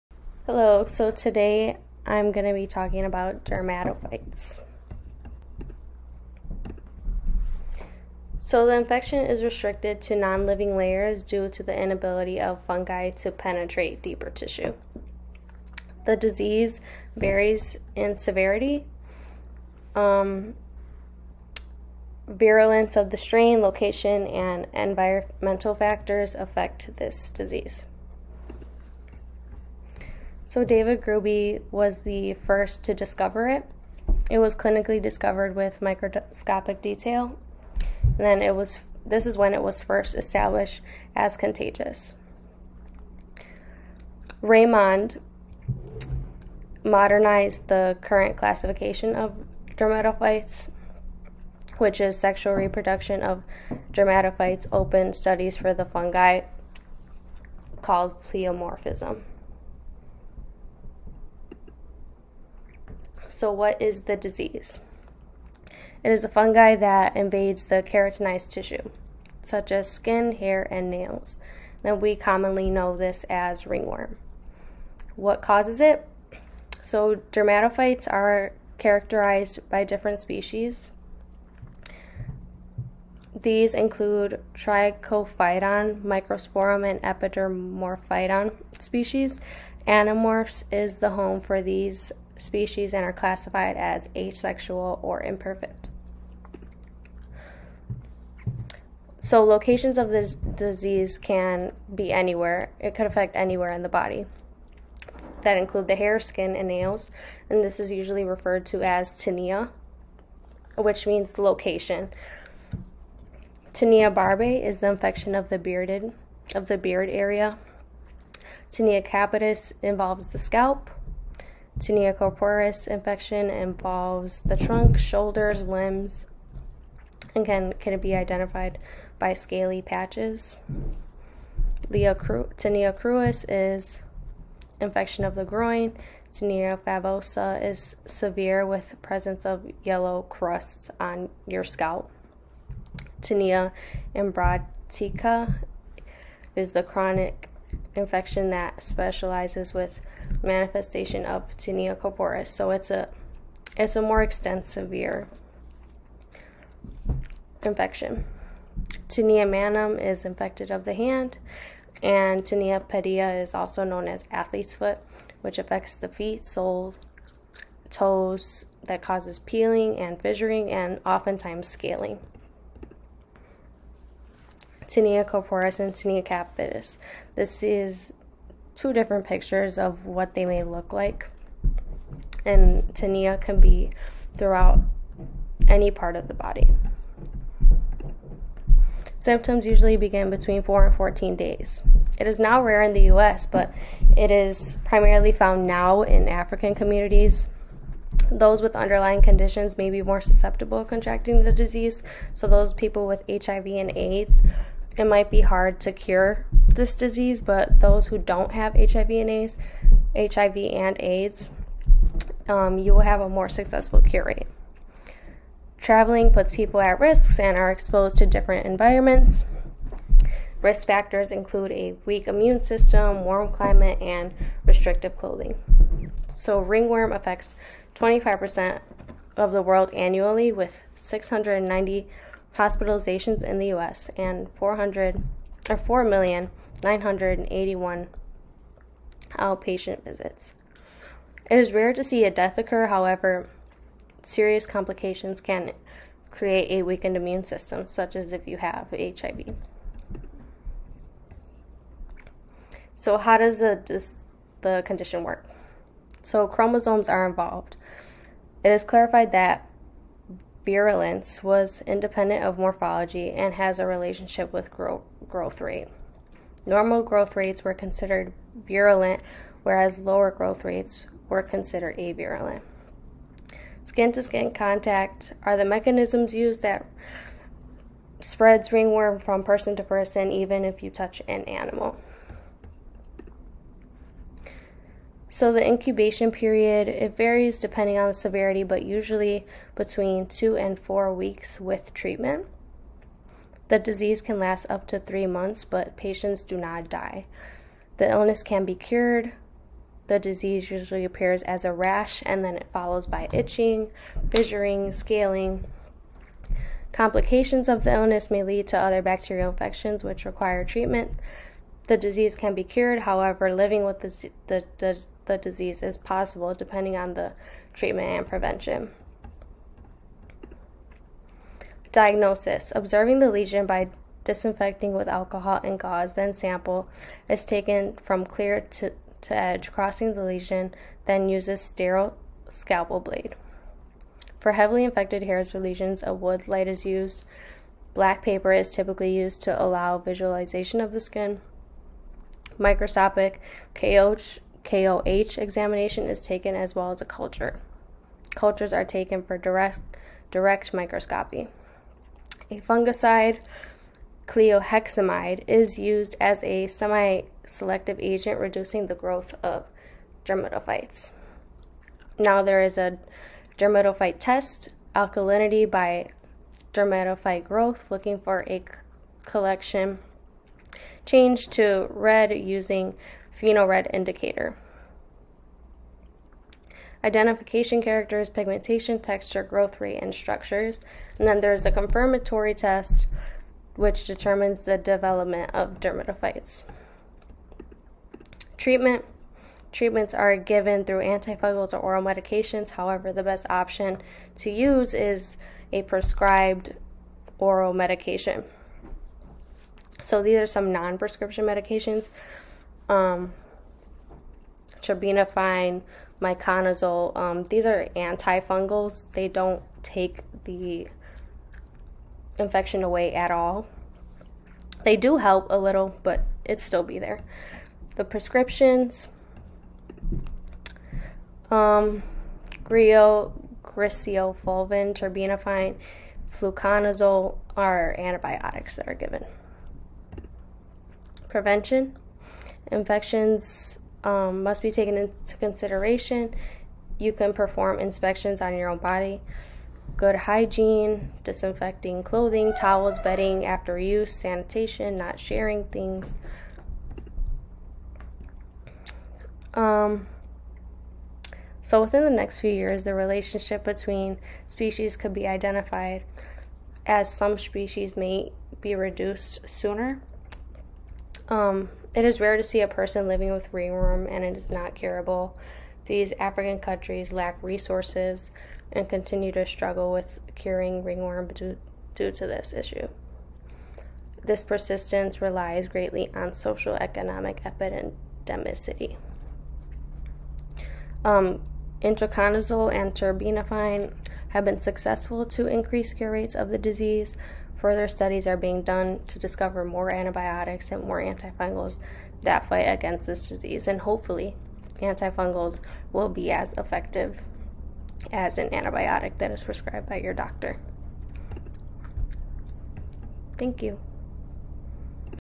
Microsoft PowerPoint - Microbiology Presentation
Hope you enjoy some information about Ringworm! My apologies for butchering some pronounciations!